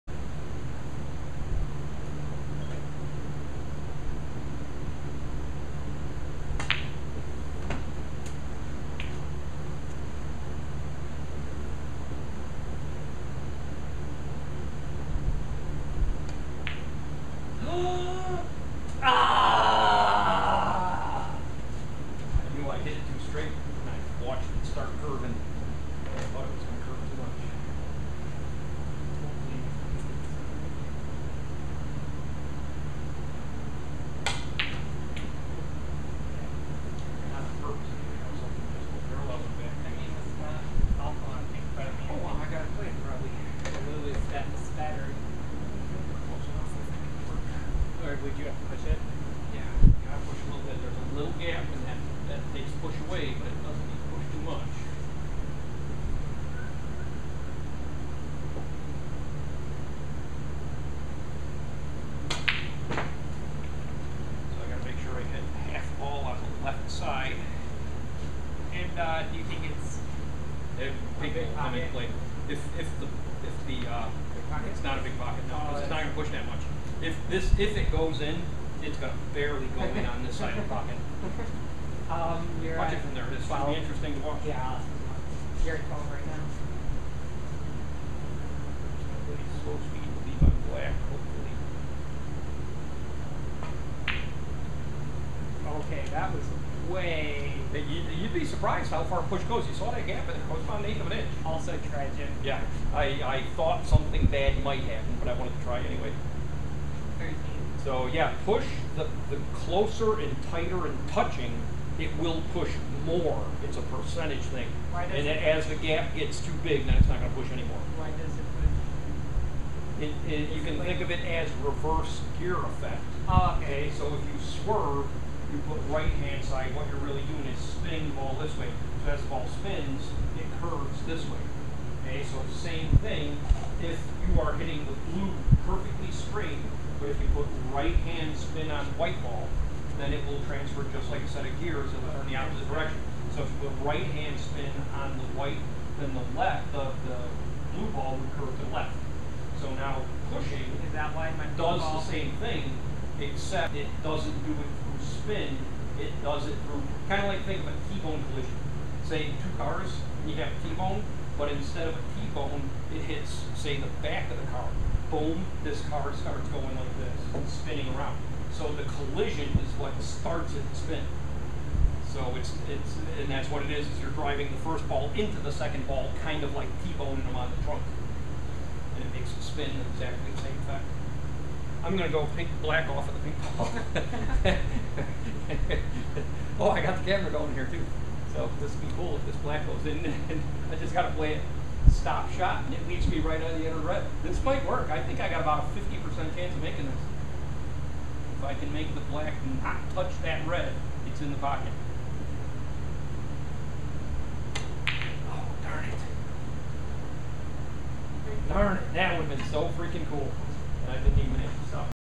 Good demo of third ball throw on a snooker table
Critical time is 0:40 through 1:40. Example of third ball contact imparting spin to the object ball to "throw" it on an unnatural line to pot what would seem to be "unpottable" plants. This effect is greatest when balls are touching, but the effect will be noticed even if there is a small gap. The "unpottable" plant begins with the Red ball nearest Pink spot. The two Reds are "naturally" lined up to a spot about an inch outside the jaw of the corner pocket OPPOSITE the Pink ball. The trick in potting these shots is knowing which side of the first plant ball to contact with the cue.

Most people who see these "throw" shots have a tendency to believe that the table must be out of level!

ACES Snooker Club northwest of Chicago
in Crystal Lake, IL